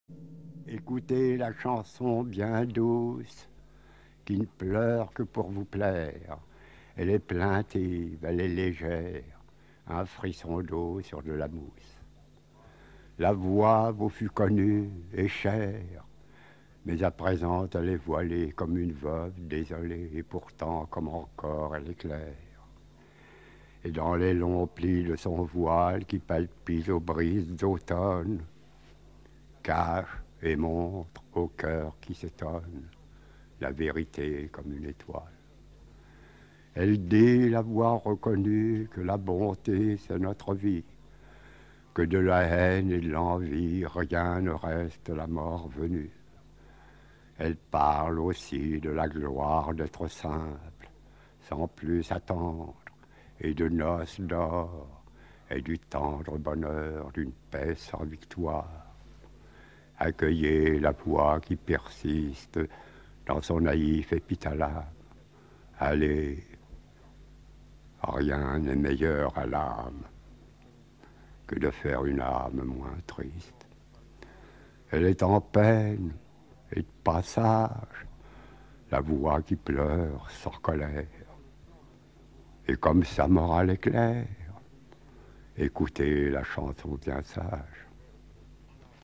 Paul Léautaud dit "Écoutez la chanson bien douce" de Verlaine
Écoutez la chanson bien douce
Qui ne pleure que pour vous plaire.
Elle est discrète, elle est légère :
Un frisson d'eau sur de la mousse !

La voix vous fut connue (et chère ?),
Mais à présent elle est voilée
Comme une veuve désolée,
Pourtant comme elle encore fière,

Et dans les longs plis de son voile
Qui palpite aux brises d'automne,
Cache et montre au coeur qui s'étonne
La vérité comme une étoile.

Elle dit, la voix reconnue,
Que la bonté c'est notre vie,
Que de la haine et de l'envie
Rien ne reste, la mort venue.

Elle parle aussi de la gloire
D'être simple sans plus attendre,
Et de noces d'or et du tendre
Bonheur d'une paix sans victoire.

Accueillez la voix qui persiste
Dans son naïf épithalame.
Allez, rien n'est meilleur à l'âme
Que de faire une âme moins triste !

Elle est "en peine" et "de passage",
L'âme qui souffre sans colère,
Et comme sa morale est claire !...
Écoutez la chanson bien sage.